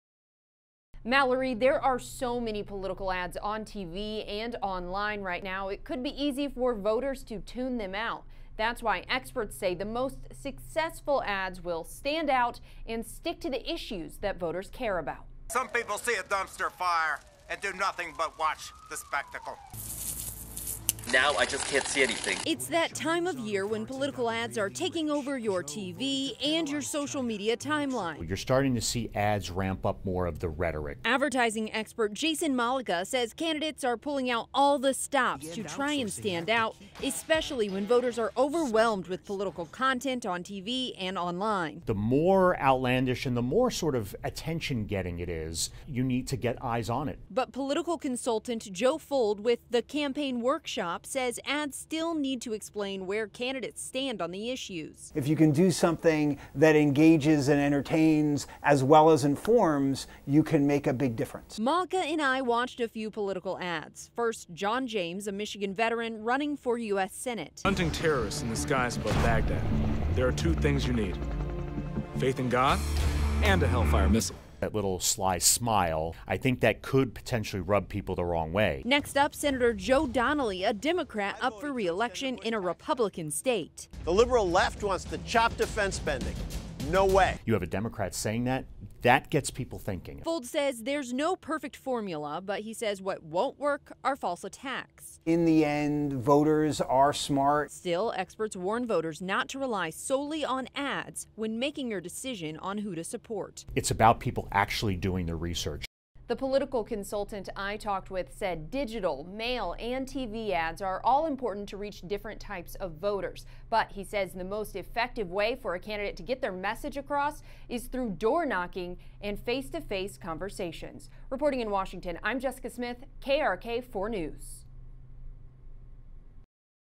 Those Crazy Political Ads
The midterms are just two weeks away and it feels like every commercial on TV is a political ad.